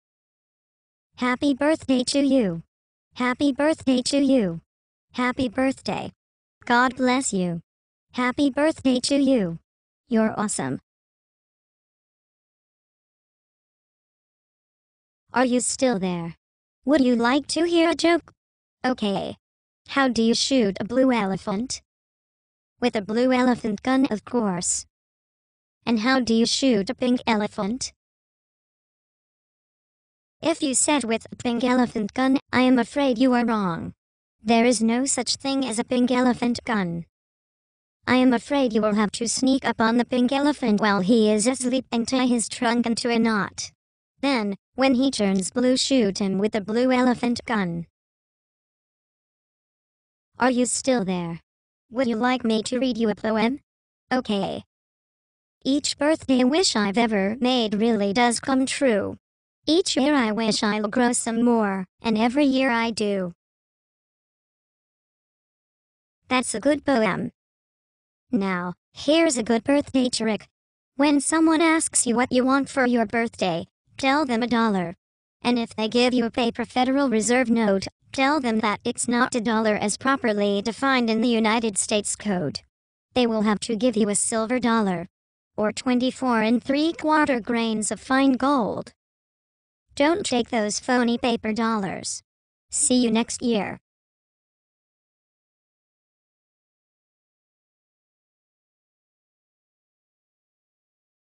Happy Birthday (Xtranormal)
Happy birthday wishes to you! He tells you a funny joke, a nice poem, and a birthday trick. Send the link to your friends for their birthday so they know you care!
Child Friendly, Rated G for GOODNESS.